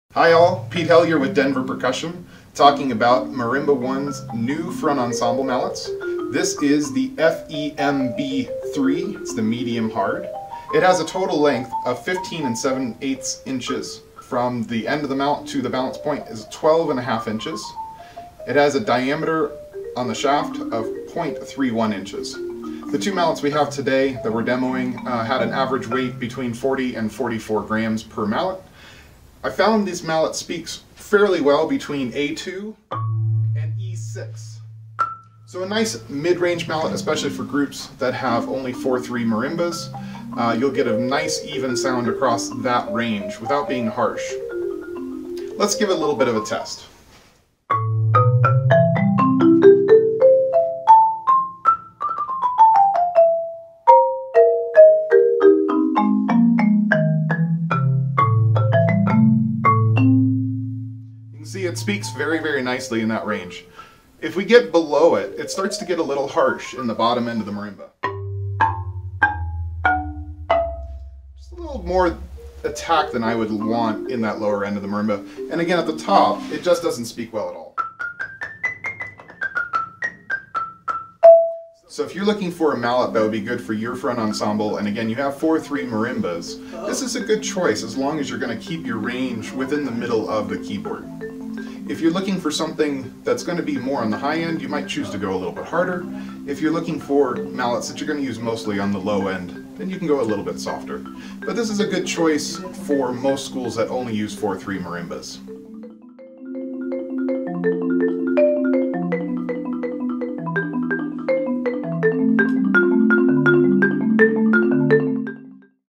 Marimba One FEMB3 Medium Hard Mallets
Marimba One Front Ensemble Mallets
FEMB3 Medium Hard Birch Mallets
Length: 15 7/8"
Balance Point: 12.5"
Diameter: .31"
Speaks from A2- E6
40-44 Grams